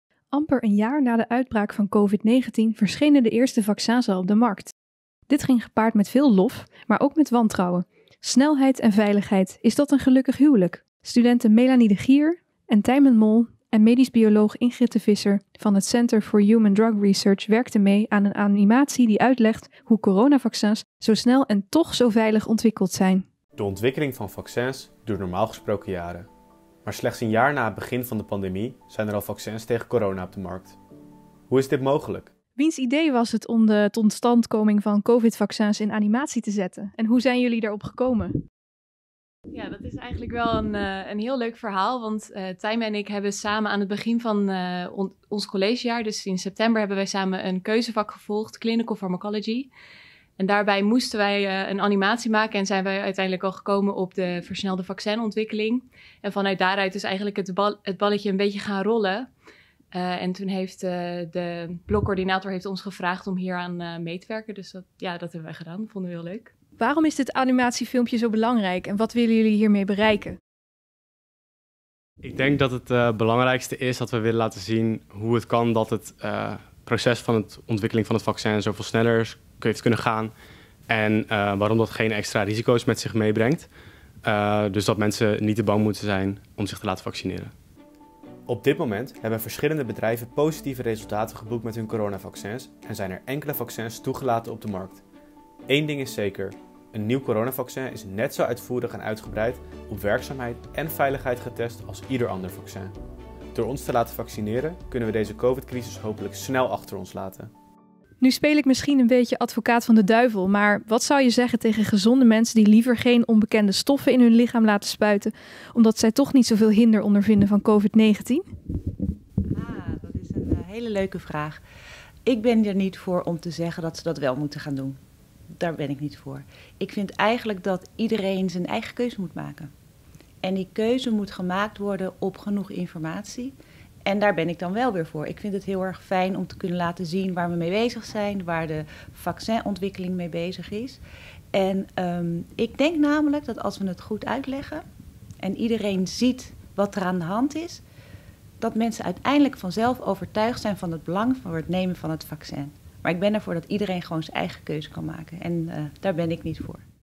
Studenten vertellen over animatie over vaccin
Studenten Melanie de Gier en Tijmen Moll vertellen samen met expert Ingrid de Visser-Kamerling over een animatie die ze gemaakt hebben over het vaccin.